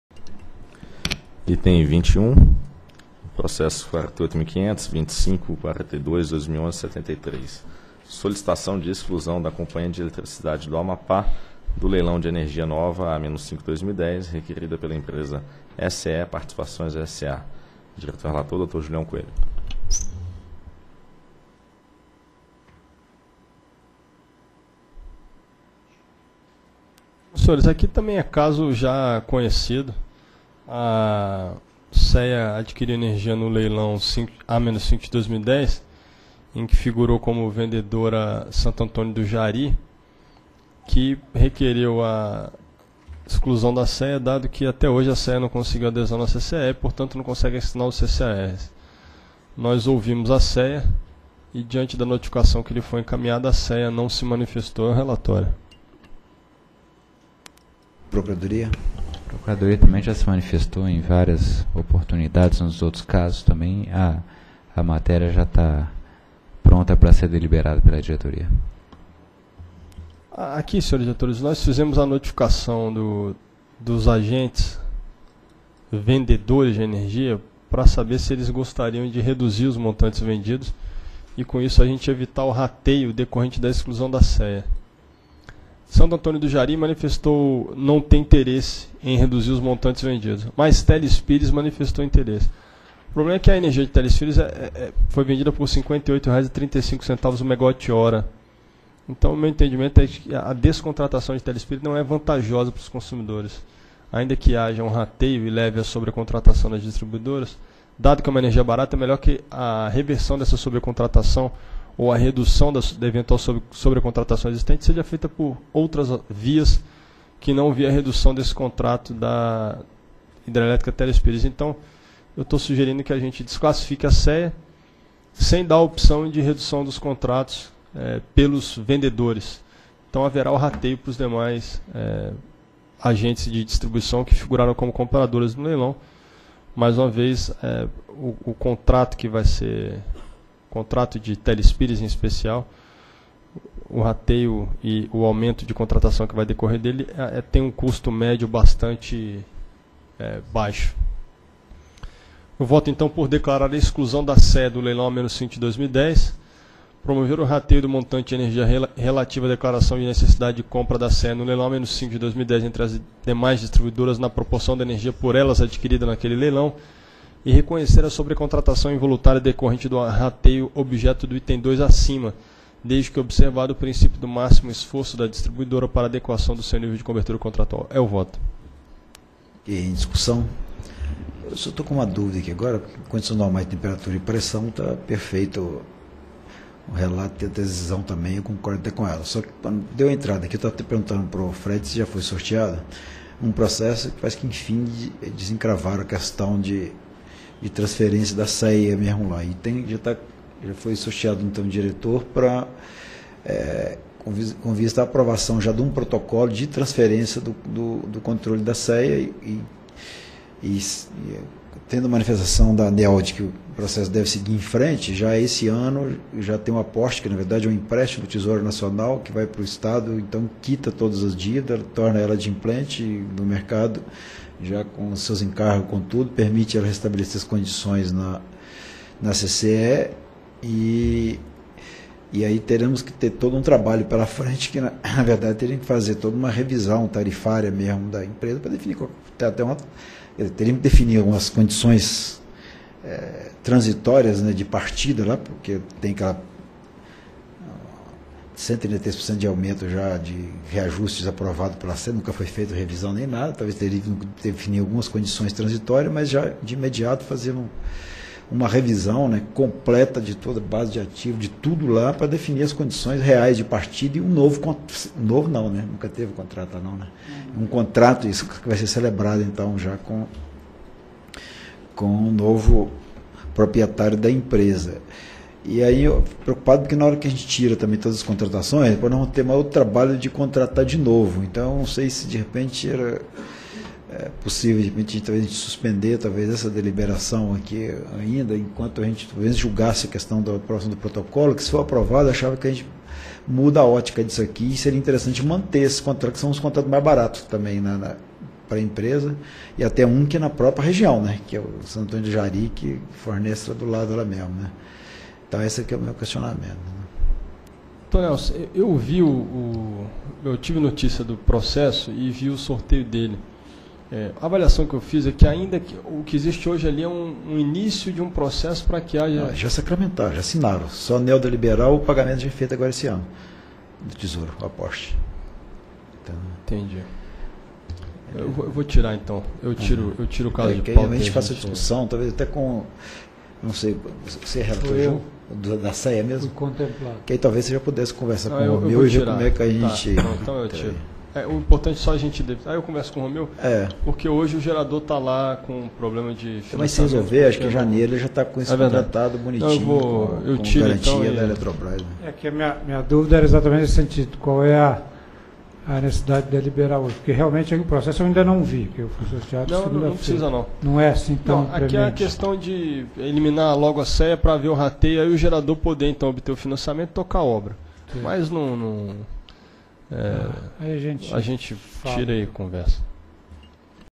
Item 21 - 47ª Reunião Pública Ordinária ANEEL 11/12/2012
Item 21. Processo: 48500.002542/2011-73. Assunto: Solicitação de exclusão da Companhia de Eletricidade do Amapá -- CEA do Leilão de Energia Nova A-5/2010, requerida pela empresa ECE Participações S.A. Relator: Diretor Julião Silveira Coelho.

Processo retirado de pauta.